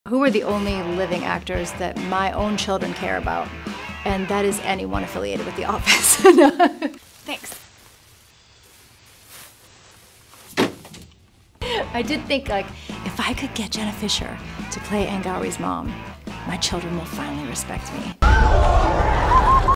Why Tina Fey Really Wanted Jenna Fischer in “Mean Girls”
Tina Fey dishes on why she wanted Jenna Fischer to star as Ms. Heron in her “Mean Girls” remake. Hint…cuz of “The Office”